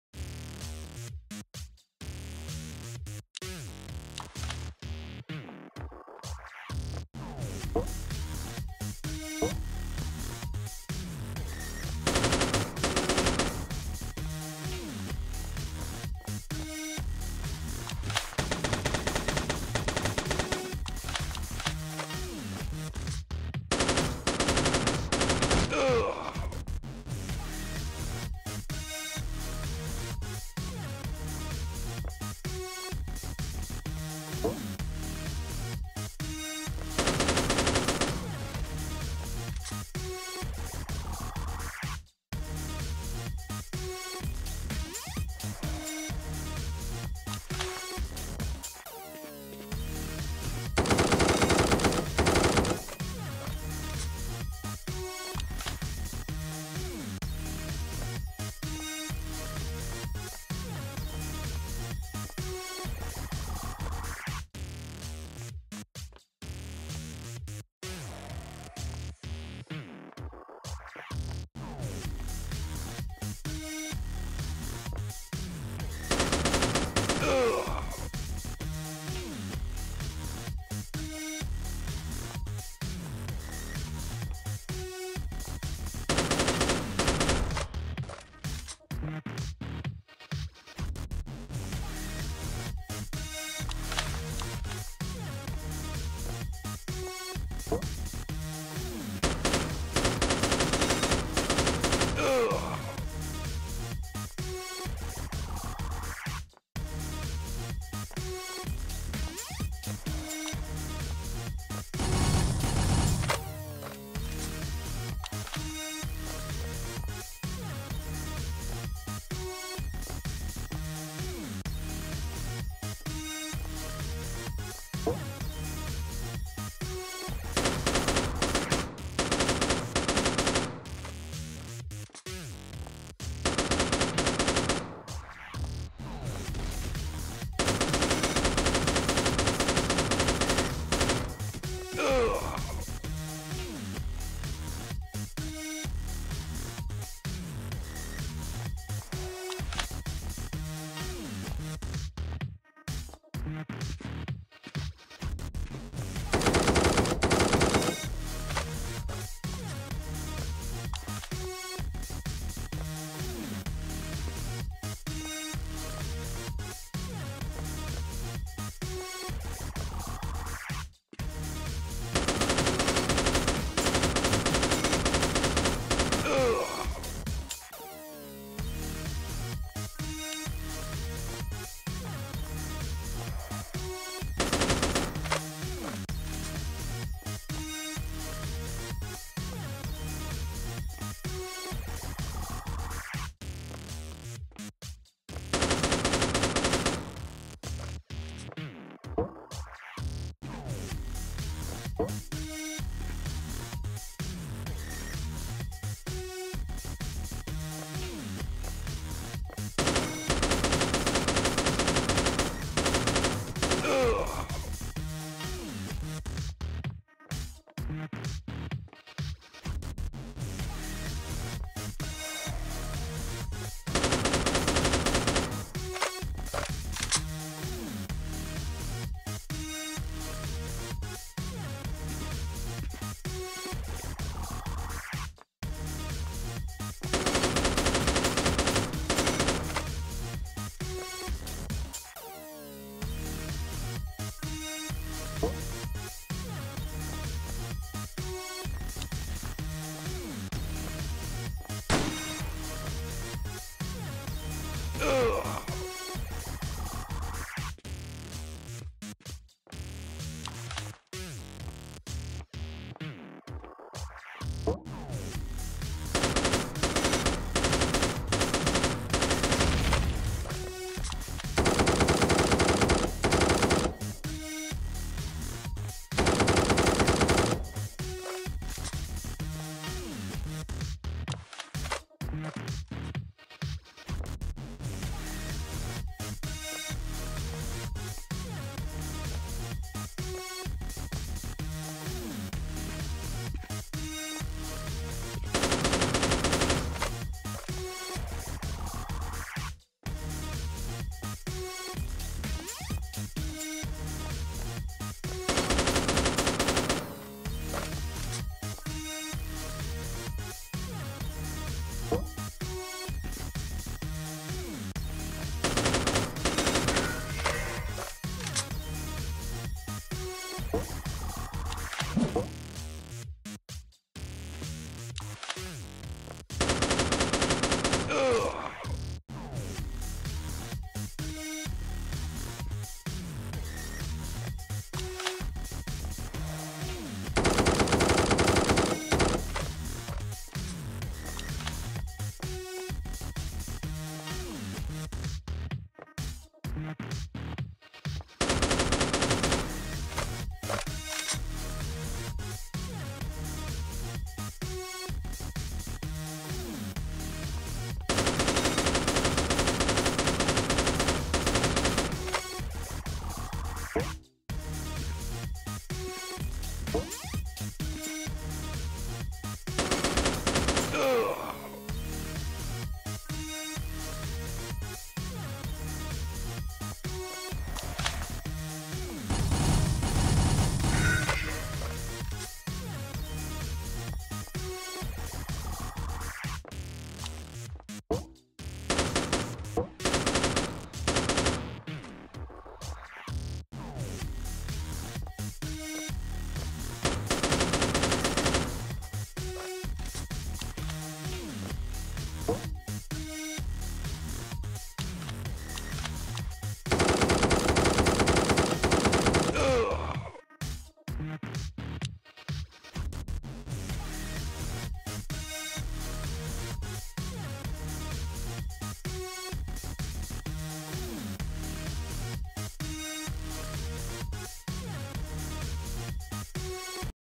1v1 - ( Krosis vs RH-SWAT ) - Cops N Robbers
This was a 1v1 between Lunar-Krosis and RH-SWAT so he can have a chance to join the legion Lunar Horizion. Watch the whole video to see who wins.